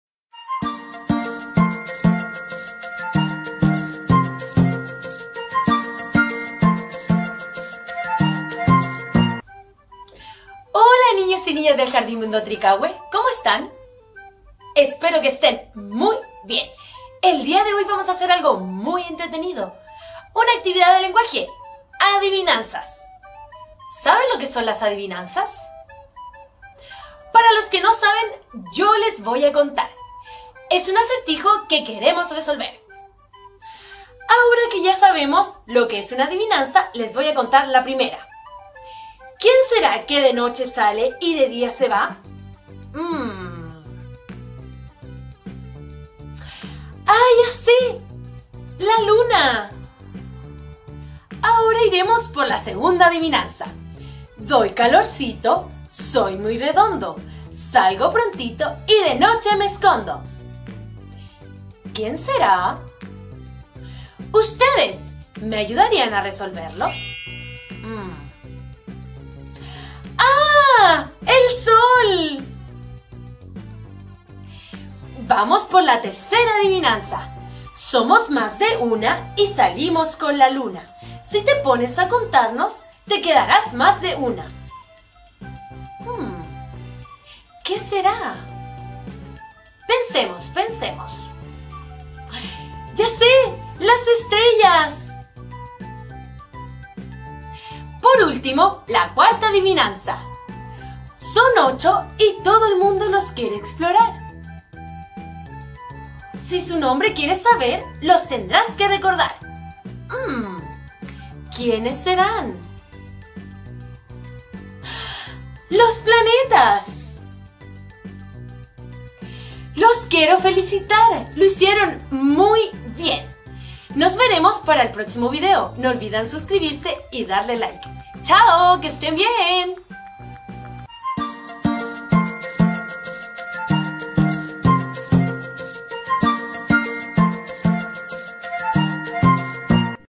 Adivinanzas del Universo para niños
Entretenidas y fácil adivinanzas relacionadas con la unidad “El Universo”